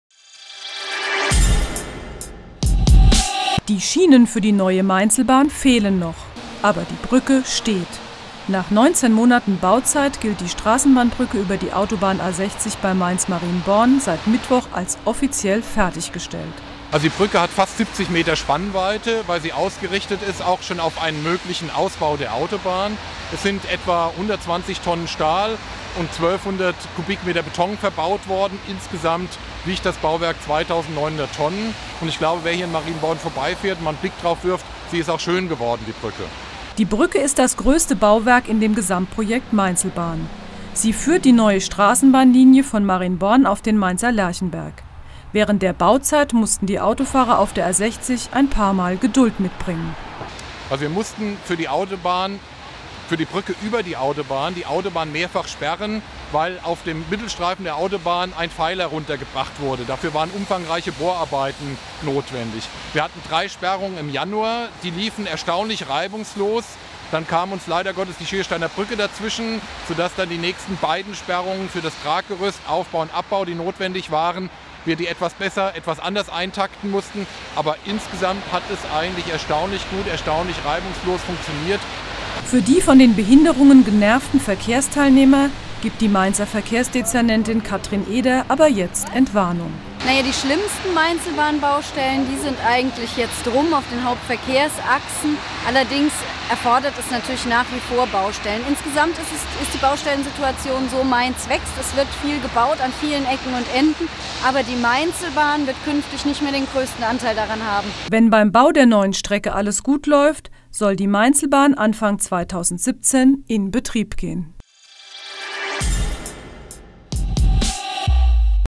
Mainzelbahnbrücke über A60 fertig
Nach 19 Monaten Bauzeit ist die Brücke für die Mainzelbahn über die A60 fertiggestellt.